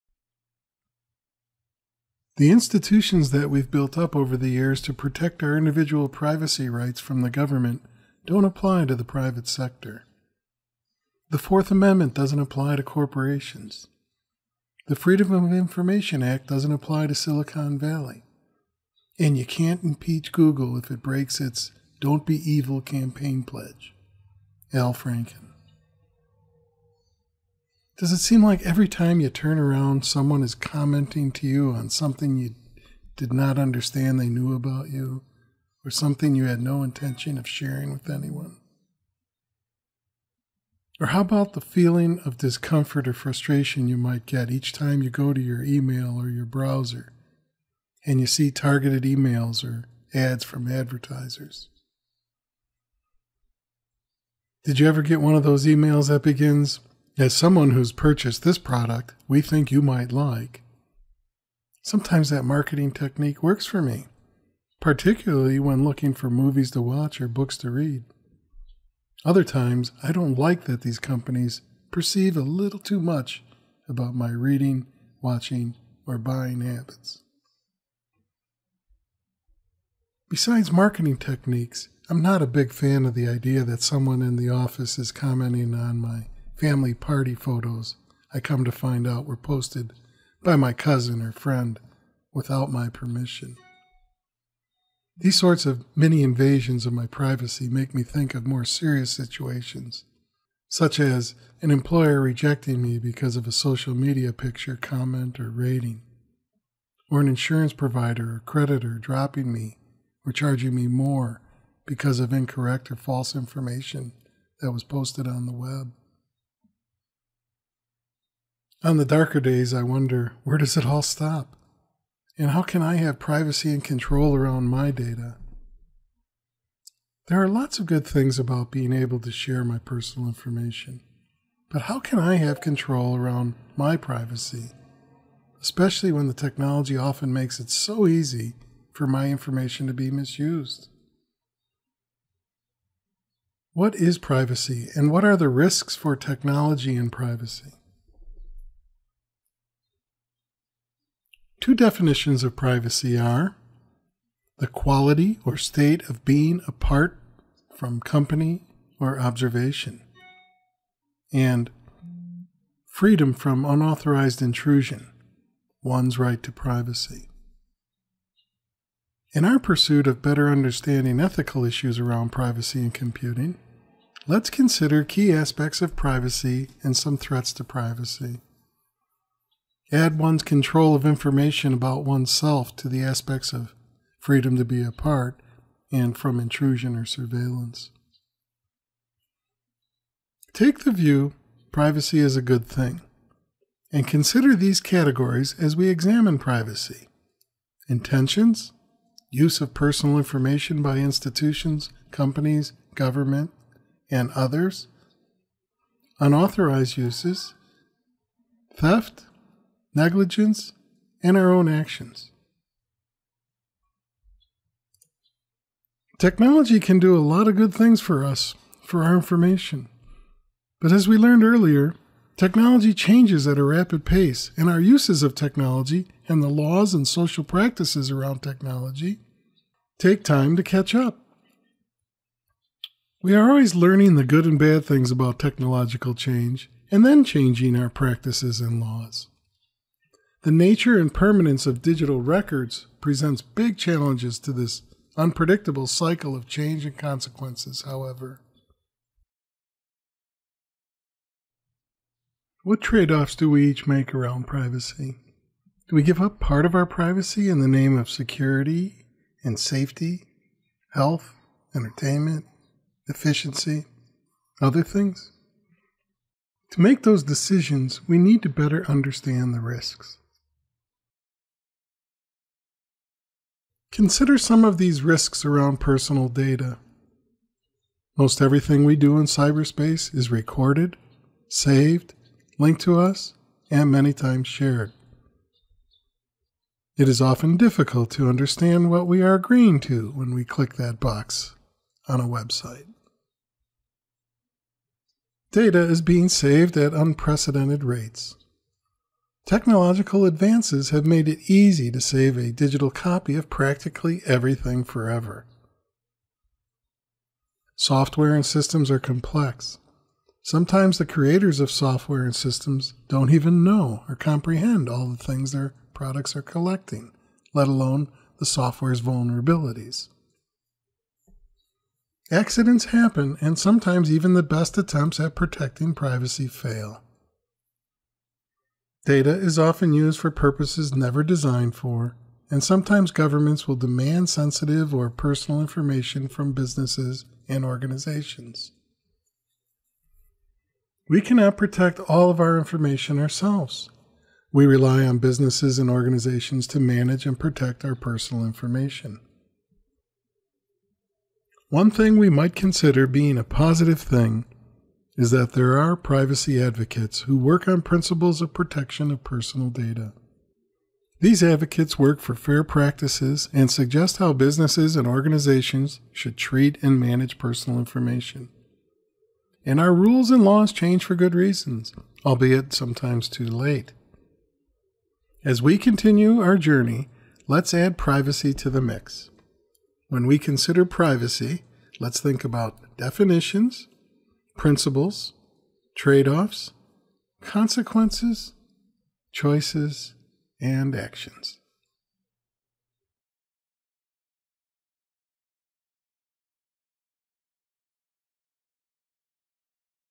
Privacy and Technological Change
What role does privacy play when technology changes?  What are things we need to consider around privacy and technological change?